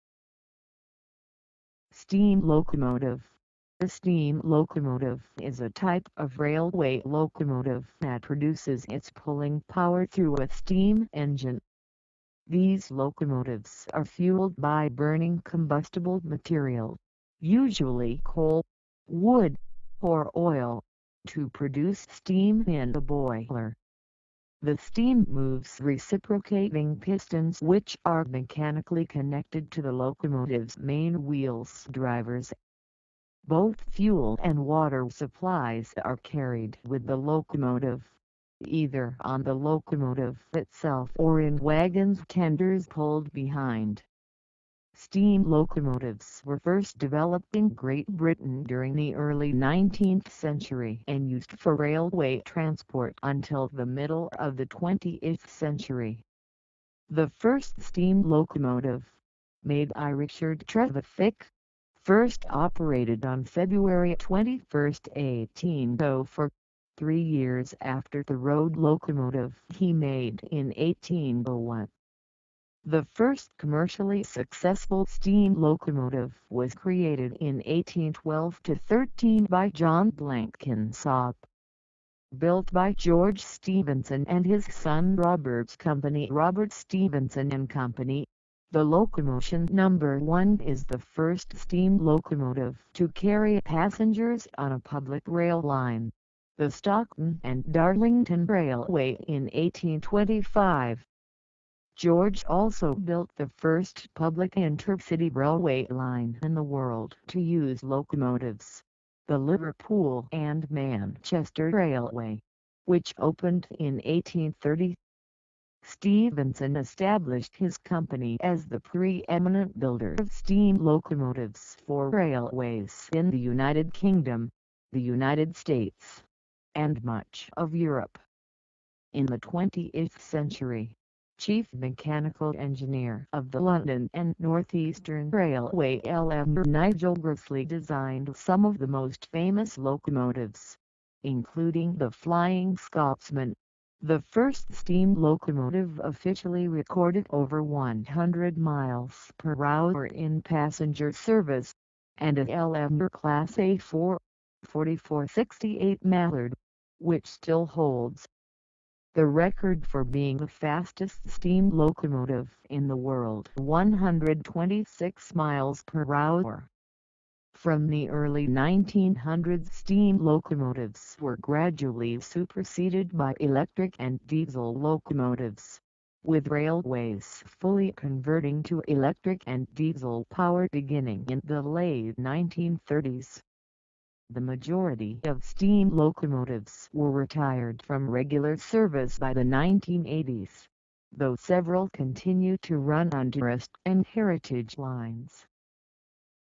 What Is STEAM LOCOMOTIVE? STEAM LOCOMOTIVE Definition & Meaning
What is STEAM LOCOMOTIVE, What does STEAM LOCOMOTIVE mean, STEAM LOCOMOTIVE meaning, STEAM LOCOMOTIVE definition, STEAM LOCOMOTIVE explanation

A steam locomotive is a type of railway locomotive that produces its pulling power through a steam engine. These locomotives are fueled by burning combustible material – usually coal, wood, or oil – to produce steam in a boiler. The steam moves reciprocating pistons which are mechanically connected to the locomotive's main wheels (drivers). Both fuel and water supplies are carried with the locomotive, either on the locomotive itself or in wagons (tenders) pulled behind.

Steam locomotives were first developed in Great Britain during the early 19th century and used for railway transport until the middle of the 20th century. The first steam locomotive, made by Richard Trevithick, first operated on 21 February 1804, three years after the road locomotive he made in 1801. The first commercially successful steam locomotive was created in 1812–13 by John Blenkinsop.[1] Built by George Stephenson and his son Robert's company Robert Stephenson and Company, the Locomotion No. 1 is the first steam locomotive to carry passengers on a public rail line, the Stockton and Darlington Railway in 1825. George also built the first public inter-city railway line in the world to use locomotives, the Liverpool and Manchester Railway, which opened in 1830. Stephenson established his company as the pre-eminent builder of steam locomotives for railways in the United Kingdom, the United States, and much of Europe.[2]

In the 20th century, Chief Mechanical Engineer of the London and North Eastern Railway (LNER) Nigel Gresley designed some of the most famous locomotives, including the Flying Scotsman, the first steam locomotive officially recorded over 100 mph in passenger service, and a LNER Class A4, 4468 Mallard, which still holds the record for being the fastest steam locomotive in the world (126 mph).[3]

From the early 1900s steam locomotives were gradually superseded by electric and diesel locomotives, with railways fully converting to electric and diesel power beginning in the late 1930s. The majority of steam locomotives were retired from regular service by the 1980s, though several continue to run on tourist and heritage lines.
Source: Wikipedia.org